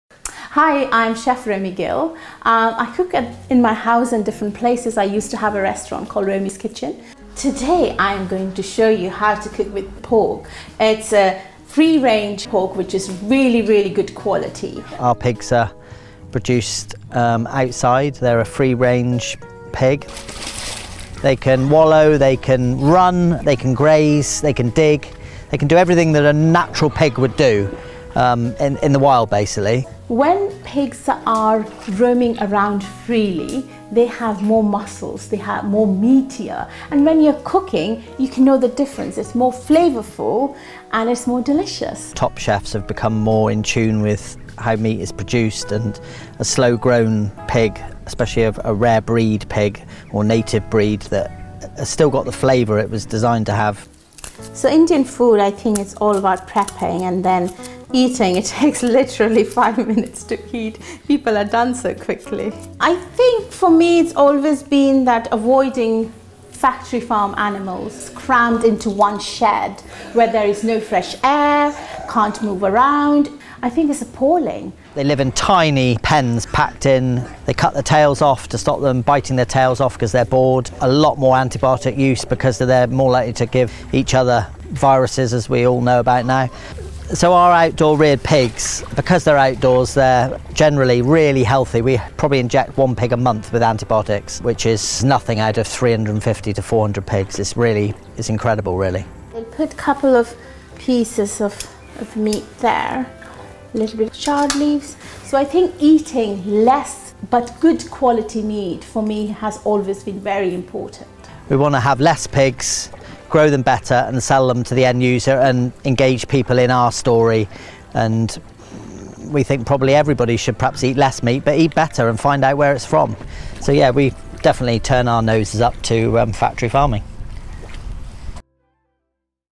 Rooting for Real Farms: Romy Gill and James Hull (The Story Pig)
👩‍🍳 Top chef, Romy Gill, was a judge on Master Chef so knows the benefits of cooking with high welfare pork sourced from farmers like James Hull 👨‍🌾. James’s herd of rare breed Tamworth pigs at The Story Pig are free range so can wallow, run, graze and root just like their wild ancestors.

James says, “Everyone should eat better meat and find out where it’s from so we definitely turn our noses up to factory farming!“